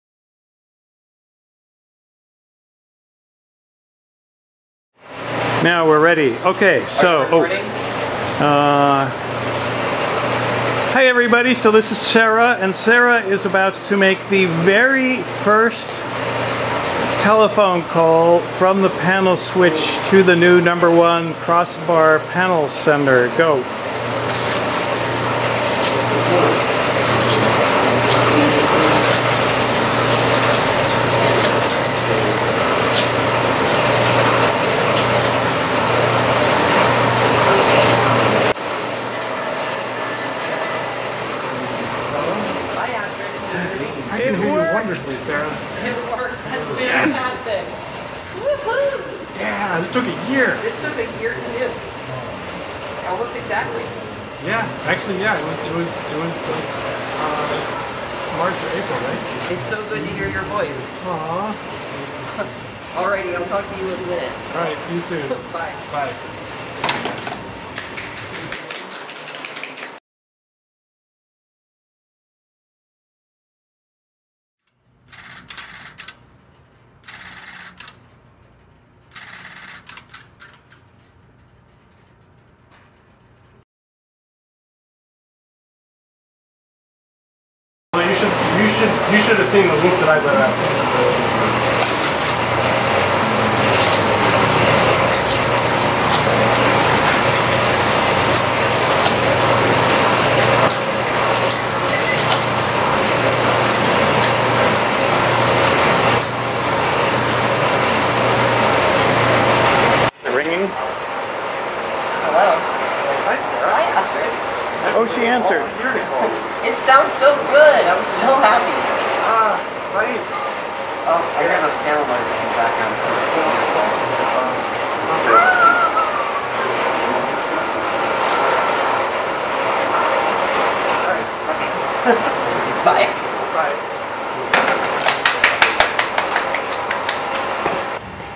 First call between Panel and Crossbar offices
This video is of the first call between our Panel and No. 1 Crossbar switches. The last time these machines were able to communicate with each other was 1974, when the Panel switch was finally decommissioned.

We did not expect this to happen today, so the video is very impromptu. Would have liked to use lav mics to capture the dialogue over the phone, but there simply wasn't any time,  since we were so excited about this.

It is my great pleasure to announce that today we made the first successful call from the Panel office to the No. 1 Crossbar!

This is a truly momentous occasion! The museum has the only remaining Panel and No. 1 Crossbar switches left in the world, and for them to once again connect calls to each other after more than 40 years is an incredible achievement!

Our Panel switch, "Rainier" served the Columbia City, and Rainier Valley neighborhoods of Seattle from the Parkway central office on Rainier Avenue, from 1923 to its replacement in 1974. The No. 1 Crossbar, "Vernon 2", served the University District from the Lakeview central office on 65th from 1942 until its replacement in the early 1980's. During their time in service, they interoperated with each other, and with the many other central offices in the city of Seattle.

After they were brought to the museum in the 1980s, communication between the two machines was never established, because the terminating senders that the "Vernon" Crossbar required to complete calls from the "Parkway" Panel office were no longer available. This has been the case ever since the museum first opened.

Last year, we were able to locate a fellow collector who had the exact parts we needed to re-establish a connection between the two machines. He helped decommission many electromechanical COs in the 1980s, and had several revertive-pulse terminating senders that he brought home from an office in Brooklyn, NY. When he heard about our machines, he enthusiastically agreed to donate the equipment we needed.

After we retrieved the units from Connecticut, a team of volunteers, young and old,  spent the last year wiring and installing them in our "Vernon" Crossbar office. It was a fantastic learning process, and we have grown together as a team during this time. I'm proud of the work we've done together, and completely humbled by the time and energy that everyone put into this project.